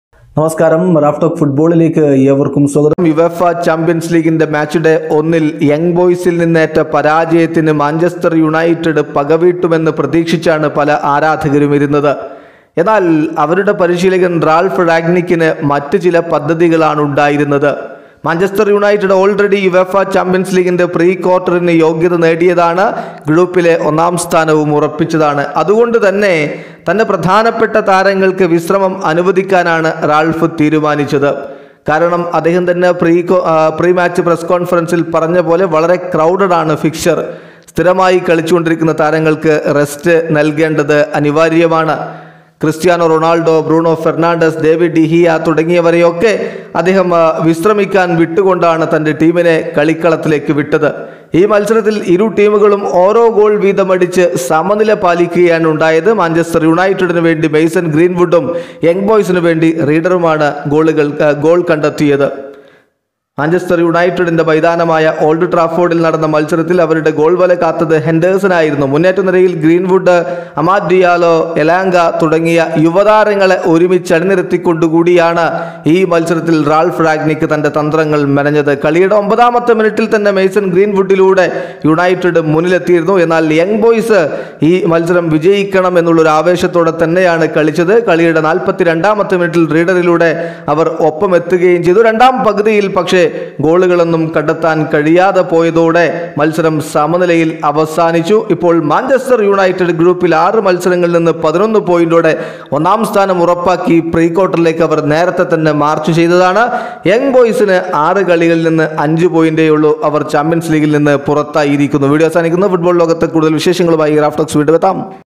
സൂപ്പർതാരങ്ങളെ പുറത്തിരുത്തി, യംഗ് ബോയ്സിനോട് പകവീട്ടാൻ യുണൈറ്റഡിനായില്ല | Man Utd vs Young Boys
Manchester United vs Young Boys